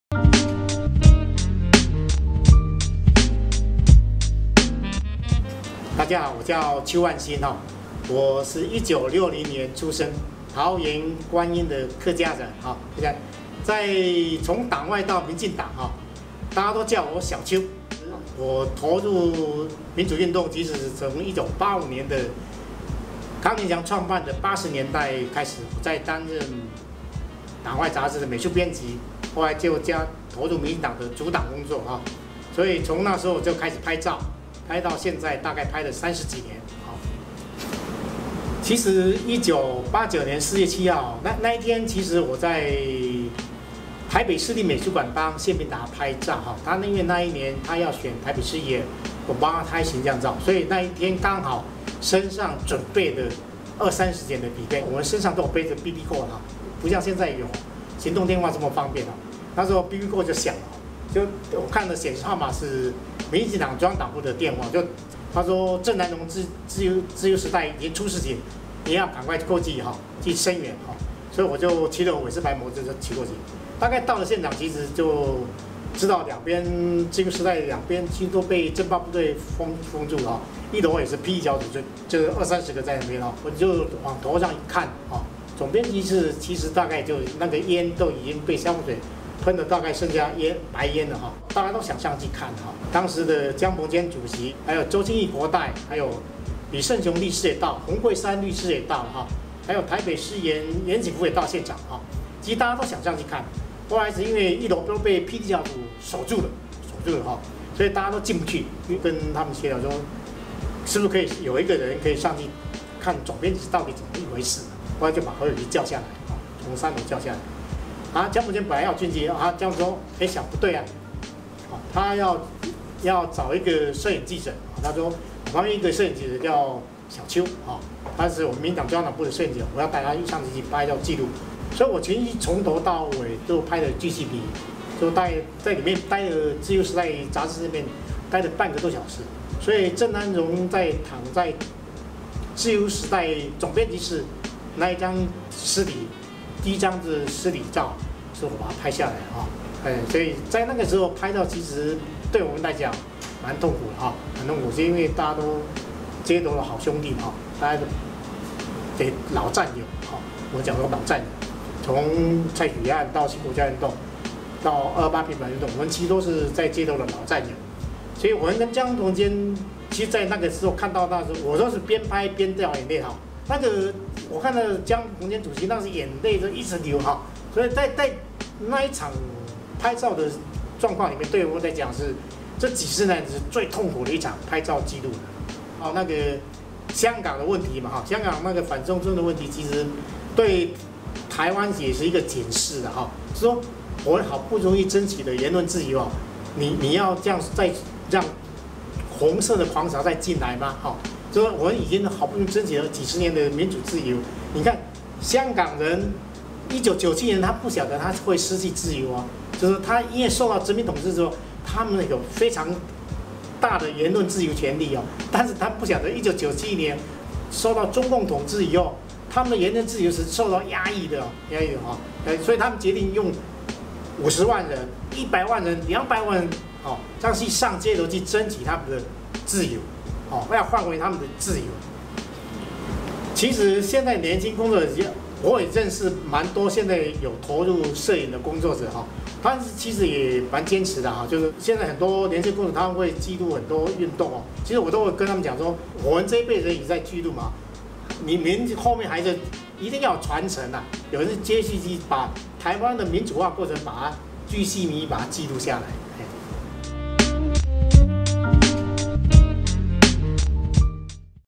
鏡頭下的台灣故事＿ TNC・人物誌＃3
邱萬興，攝影師。
認識他的前輩都叫他「小邱」

三十多年來，這片土地上大大小小的遊行、抗議，
都有他的身影。

從底片，到數位。
他用手中的相機，紀錄民主的瞬間。

這是他的故事。